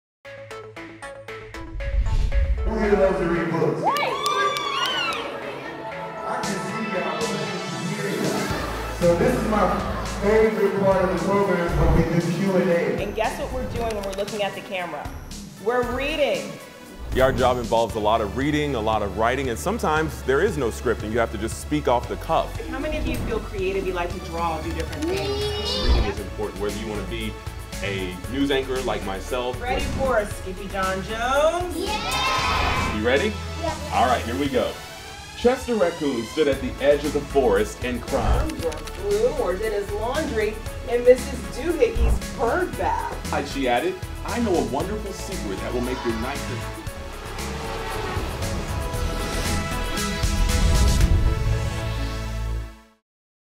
Melinda & Jonathan read for The Wade Smith Foundation
The Wade Smith Foundation's Reading with the Pros Literacy Tour promotes and supports childhood literacy and education through in-person celebrity book readings and classroom visits.